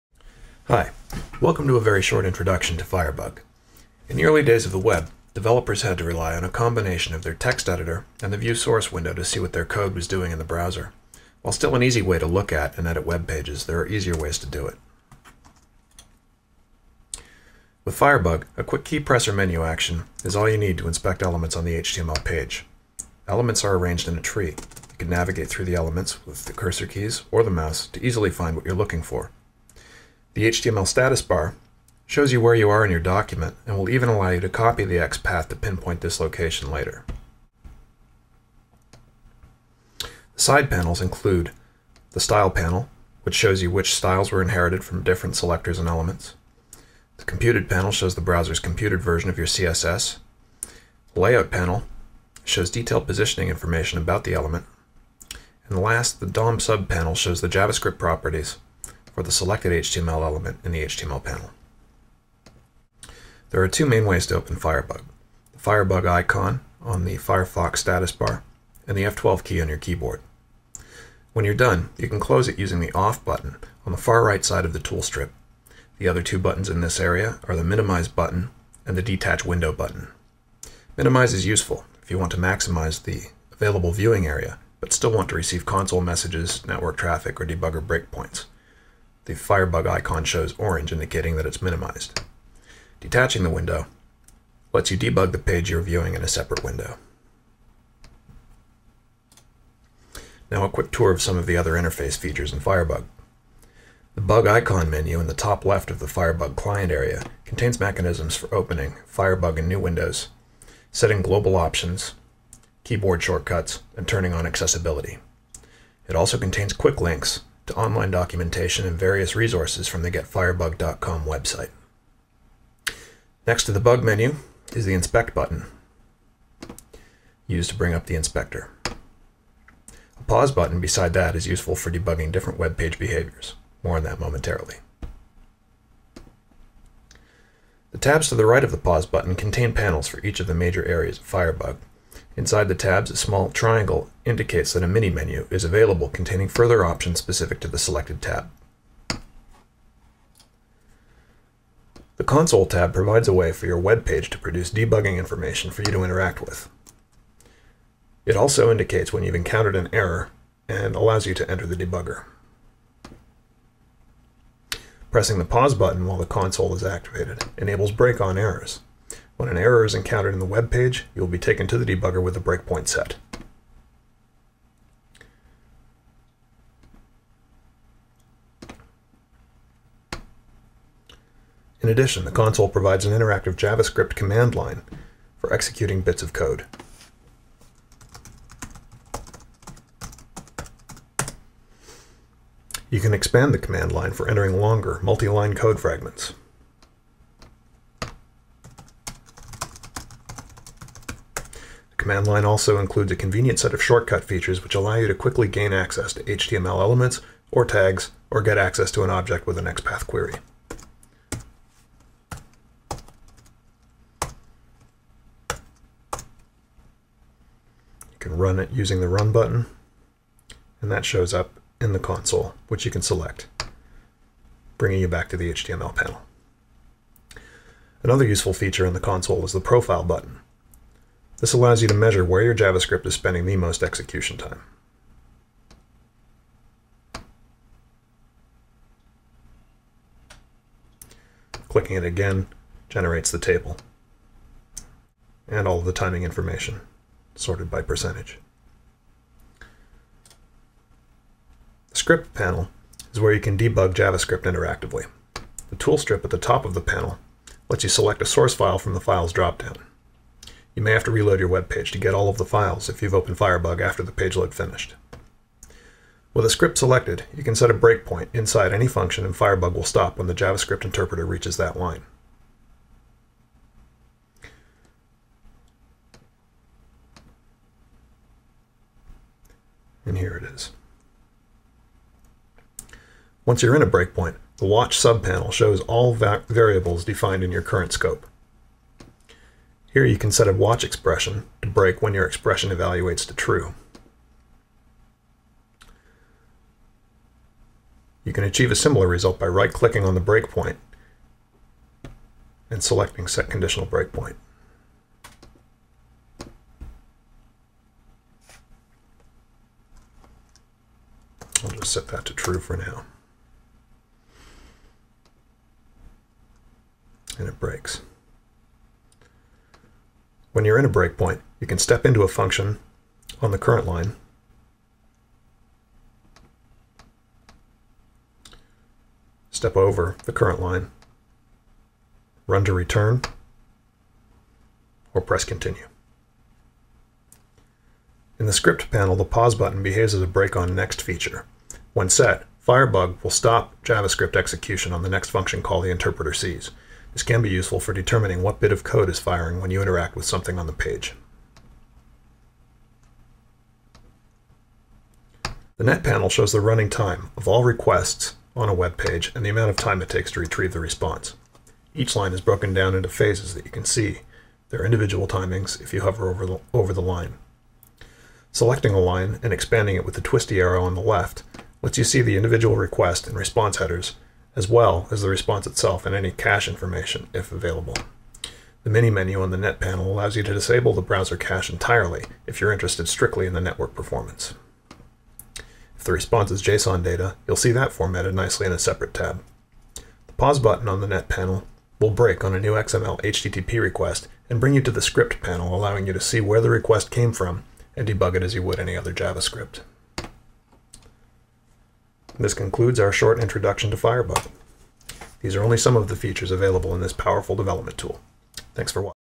Introduction to Firebug
Chief Firebug pyroentomologist Rob Campbell gives a quick introduction to Firebug. Download Firebug at getfirebug.com